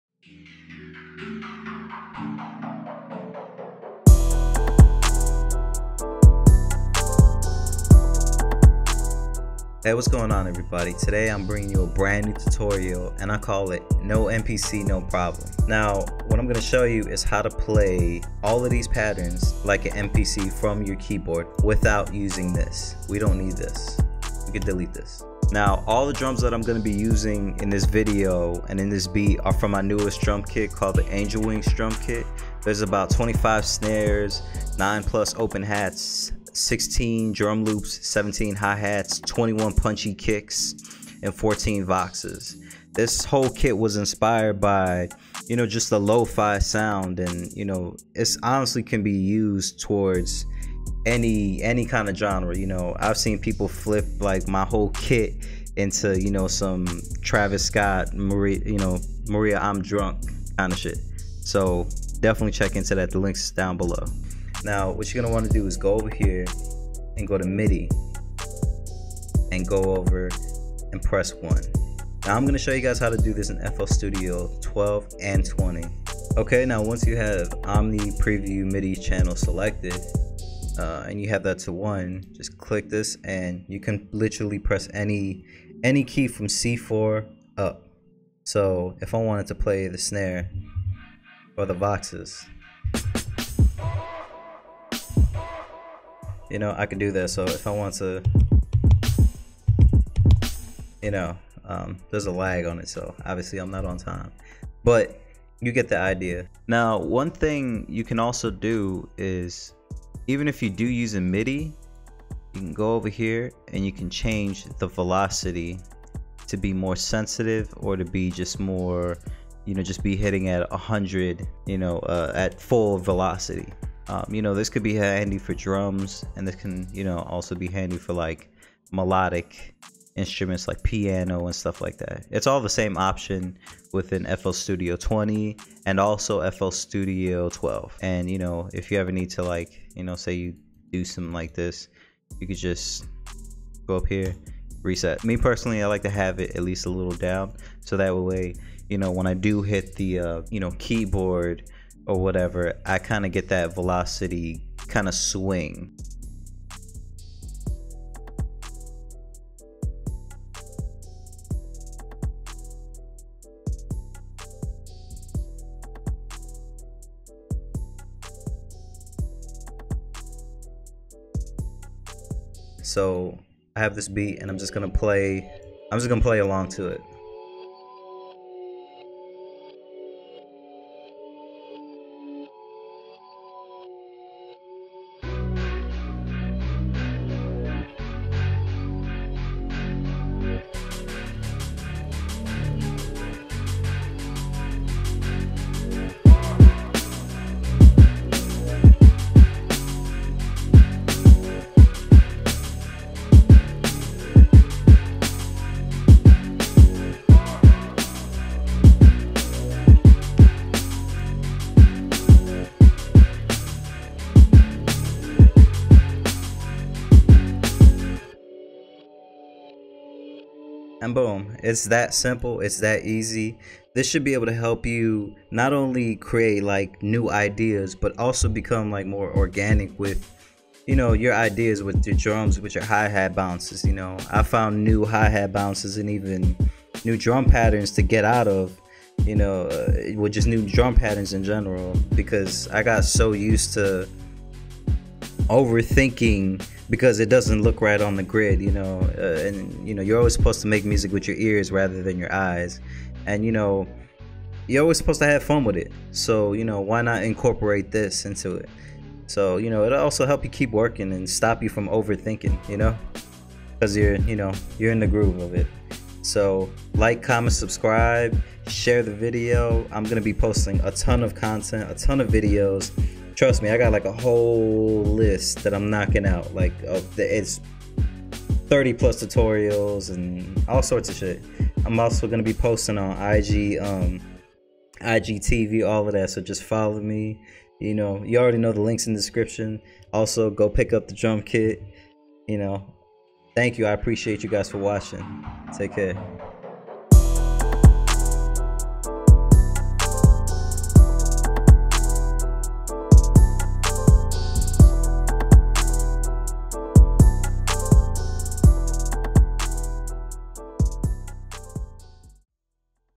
NO MPC NO PROBLEM! HOW TO USE YOUR KEYBOARD AS AN MPC WITHOUT PLUGINS !! FL STUDIO TUTORIAL
NO MPC NO PROBLEM! HOW TO USE YOUR KEYBOARD AS AN MPC WITHOUT PLUGINS !!